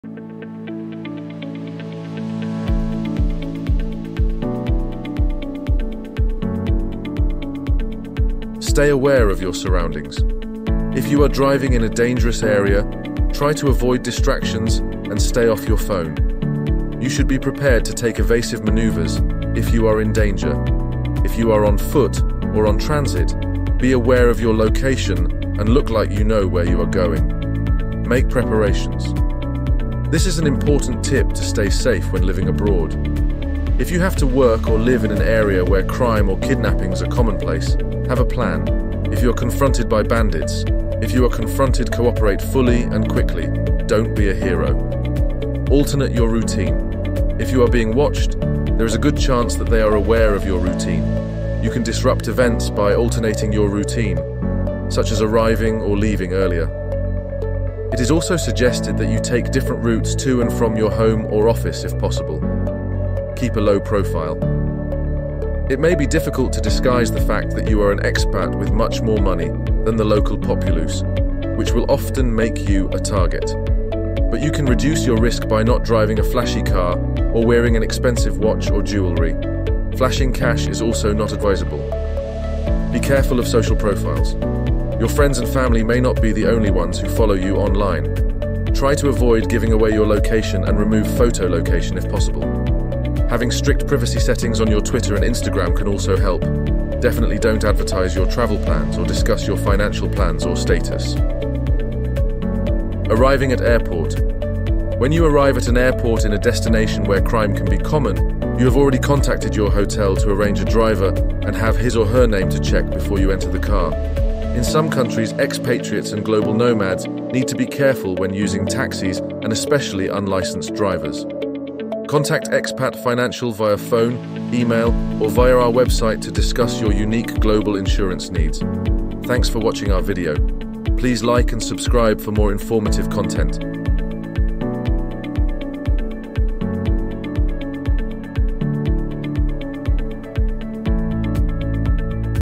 Safe and Sound Abroad: 6 Essential Tips for International Living
Are you planning to live abroad or already an expat soaking in the global experience? Safety is paramount, and in this video, we've got you covered with six indispensable tips to keep you secure while living in a foreign land. From understanding local customs to emergency preparedness, these practical insights will ensure your time abroad is not only enjoyable but also safe. Whether you're a digital nomad, student, or professional, these tips are universally applicable. Join us in this video to gain valuable insights and make your international journey a secure and memorable one.

Watch now and empower yourself with the essential safety tips for living abroad!